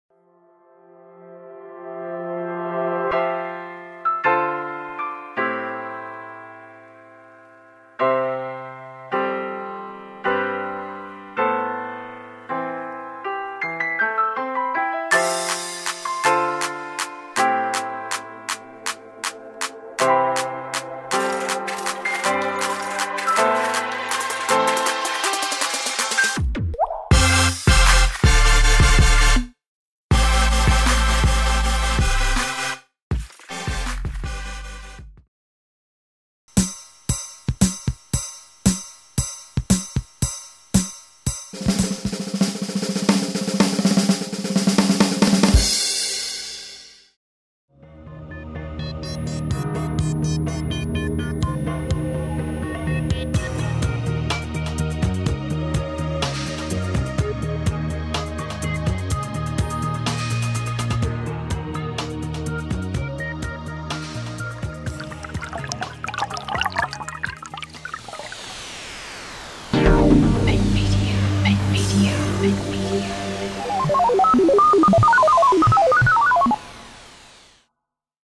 Final Cut Beispiele
In den Makemedia Studios des LIS können Bremer Schüler*innen Videoschnittplätze nutzen und mit der Schnittsoftware Final Cut X arbeiten. Wir haben für Euch einen kleinen Film produziert um zu zeigen, was man mit Final Cut so machen kann...